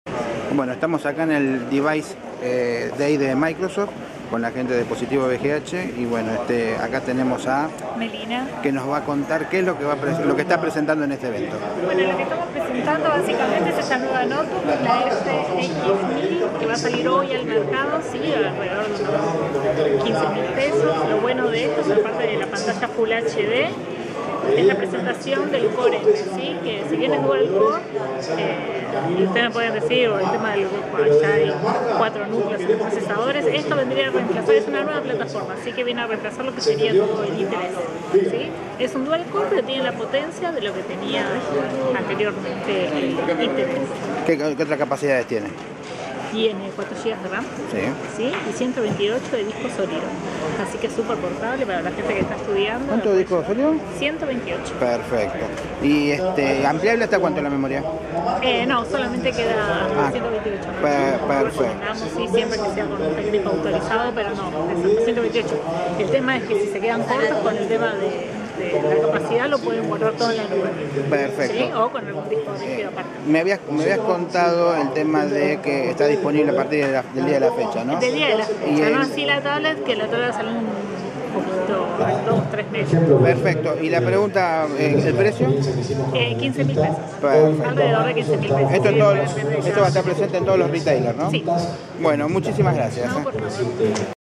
PositivoBGH presente en el DevicesDay de Microsoft
Visitamos el stand de Positivo BGH, que nos mostraron las novedades que presentaron en el DevicesDay Microsoft

Seguinos en Twitter: @itenunclick, @gabybiondi.